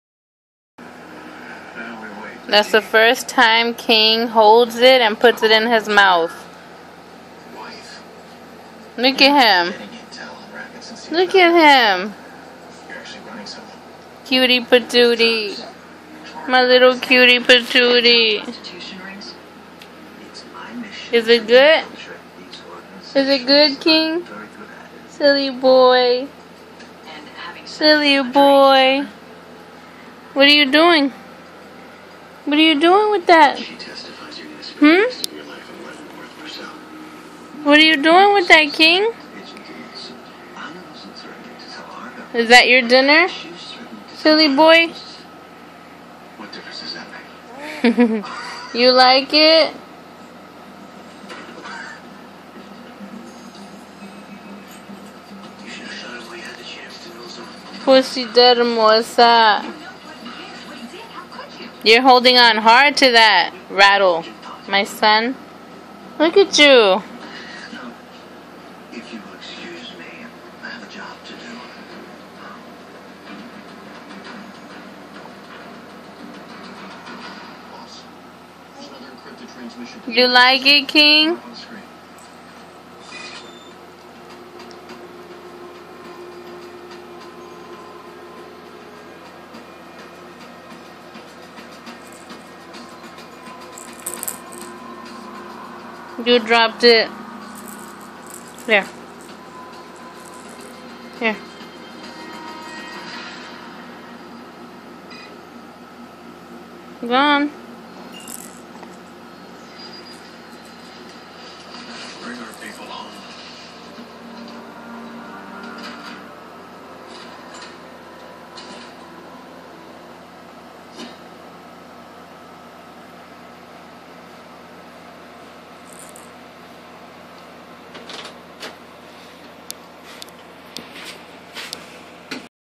Kings baby Rattle
Kingstons first time holding tight to his rattle with two hands and aiming it to his mouth.